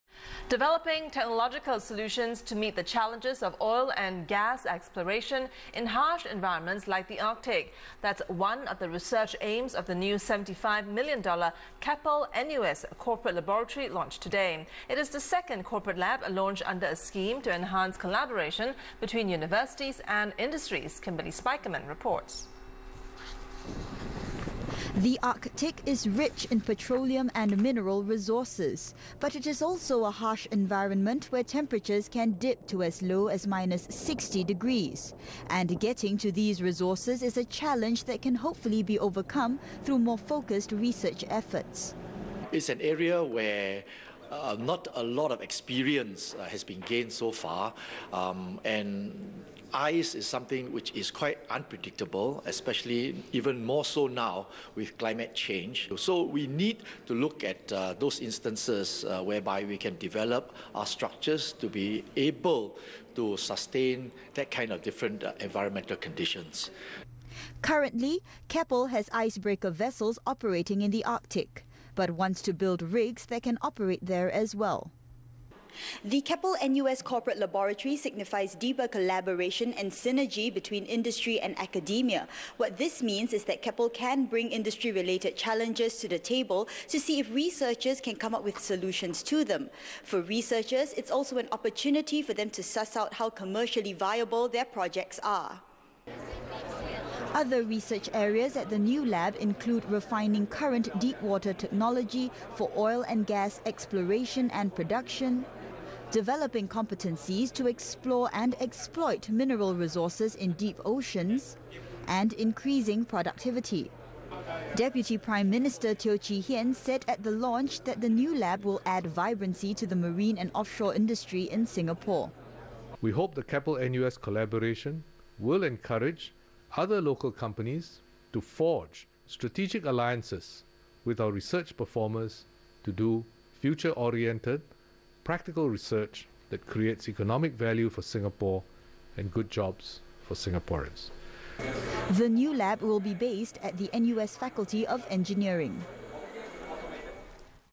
New corporate laboratory for marine and offshore sector - 25Nov2013
SINGAPORE: The new S$75 million Keppel-NUS Corporate Laboratory was launched in Singapore on Monday.

One of its research aims is to develop technological solutions to meet the challenges of oil and gas exploration and production in harsh environments like the Arctic.

It is the second corporate laboratory launched under a scheme by the National Research Foundation to enhance collaboration between universities and industries.

The first - the Rolls-Royce@NTU Corporate Lab - was launched in July 2013. The Keppel-NUS Corporate Lab is the first involving a local company.

The Arctic is rich in petroleum and mineral resources. But it is also a harsh environment where temperatures can dip to as low as minus 60 degrees.

Getting to these resources is a challenge that can hopefully be overcome through more focused research efforts.

Michael Chia, managing director (Marine and Technology) at Keppel Offshore & Marine, said: "It is an area where not a lot of experience has been gained so far. And ice is something which is quite unpredictable, especially even more so now with climate change... So we need to look at those instances whereby we can develop our structures to be able to sustain that kind of different environmental conditions."

Currently, Keppel has icebreaker vessels operating in the Arctic, but it wants to build rigs that can operate there as well. It hopes to deploy arctic jack-up drilling rigs there by 2020.

The Keppel-NUS Corporate Laboratory signifies deeper collaboration and synergy between industry and academia.

This means that Keppel can bring industry-related challenges to the table to see if researchers can come up with solutions to them. For researchers it is also an opportunity for them to suss out how commercially viable their projects are.

Other research areas at the new lab include refining current deepwater technology for oil and gas exploration and production, developing competencies to explore and exploit mineral resources in deep oceans in an environmentally-friendly way, and increasing productivity while reducing reliance on manpower in some shipyard activities such as welding, painting and operations in confined spaces.

Professor Low Teck Seng, CEO of the National Research Foundation, said: "I think what is important for us in Singapore is to make sure the investments we have made in research and development get translated so that there is accrued benefit to Singapore and Singaporeans.

"So this aspect of translation can only be effected if you bring companies together with our research performers, the universities and research institutes. So having this Keppel corporate lab in NUS is a very pro-active approach in seeing how we can actually catalyse and accelerate translation of technologies in a very important area for us, because Keppel is number one in certain areas of technology in the marine-offshore space."

Deputy Prime Minister Teo Chee Hean said at the launch that the new lab will add vibrancy to the marine and offshore industry in Singapore.

Mr Teo, who is also chairman of the National Research Foundation, said: "We hope the Keppel-NUS collaboration will encourage other local companies to forge strategic alliances with our research performers, to do future-oriented, practical research that creates economic value for Singapore and good jobs for Singaporeans."

The partnership between Keppel Corporation and NUS will involve 44 principal investigators, co-principal investigators and external collaborators.

It will also create 54 new positions for research fellows and engineers and train some 30 PhD and Masters students in five years.

Today, Singapore's marine and offshore industry contributes S$13 billion, or 1.6 per cent, to the gross domestic product, and hires some 20,000 Singaporeans and permanent residents, mainly in skilled jobs.

The new lab will be based at the NUS Faculty of Engineering.